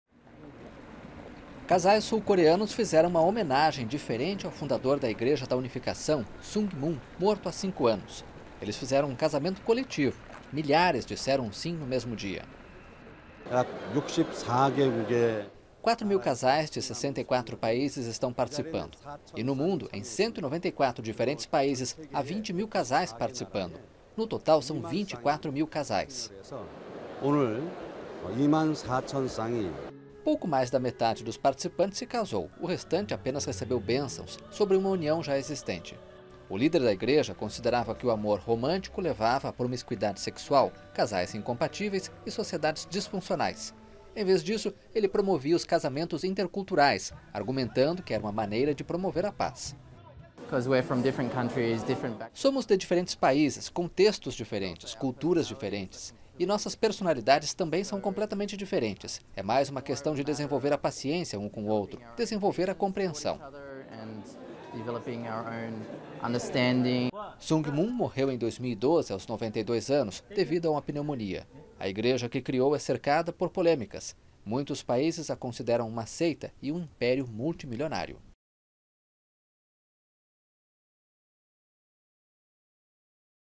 Milhares casam na Coreia do Sul
Milhares de casais disseram o sim em um casamento coletivo na Igreja da Unificação. Foi uma forma de celebrar o quinto aniversário da morte do fundador da igreja, Sun Myung Moon.